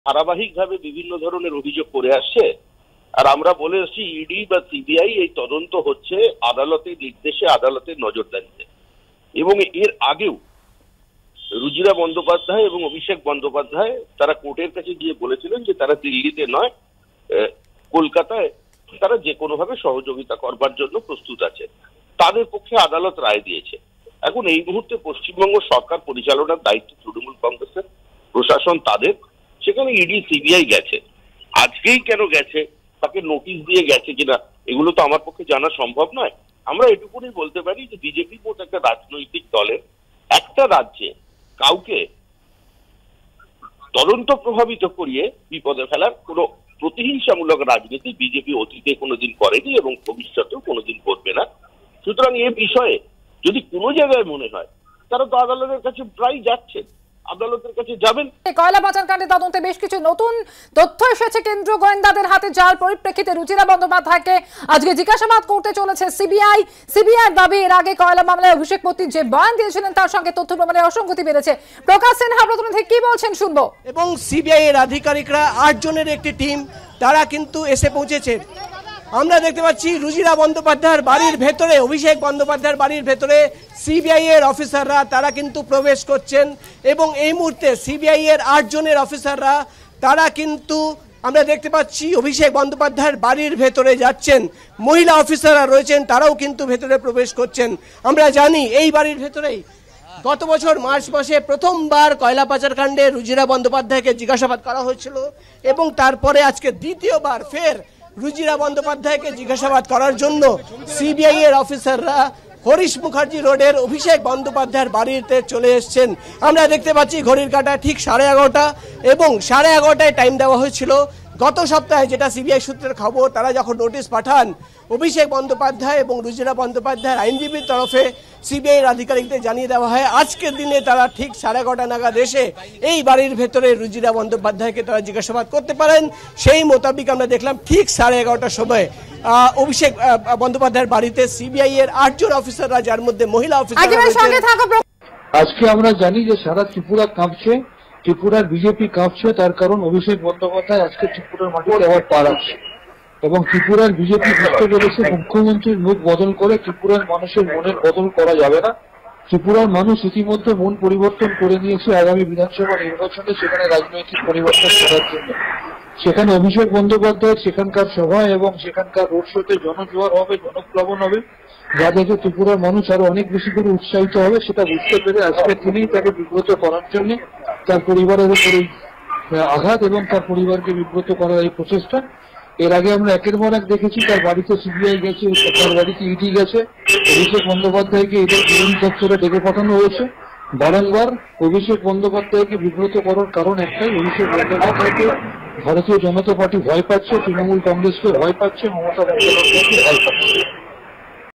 Samik Bhattachariya: রুজিরা বন্দ্যোপাধ্যায়কে জিজ্ঞাসাবাদ করবে সিবিআই।কী প্রতিক্রিয়া শমীক ভট্টাচার্যর
কয়লা পাচারকাণ্ডে রুজিরা বন্দ্যোপাধ্যায়কে আজই জিজ্ঞাসাবাদ করবে সিবিআই। কী প্রতিক্রিয়া শমীক ভট্টাচার্যর?